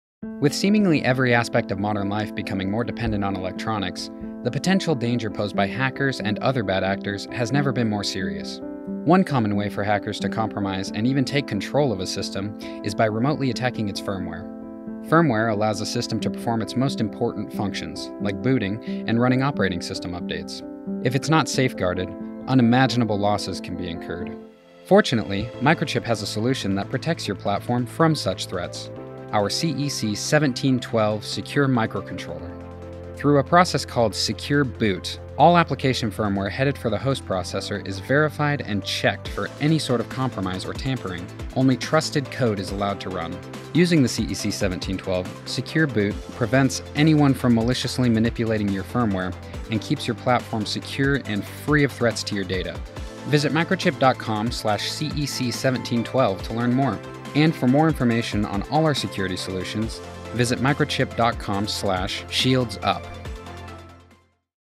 CEC1712 Secure Boot Introduction
With seemingly every aspect of modern life becoming more dependent on electronics, the potential danger posed by hackers and other bad actors has never been more serious.
Fortunately, Microchip has a solution that protects your platform from such threats, our CEC1712 secure microcontroller.

to learn more, and for more information on all our security solutions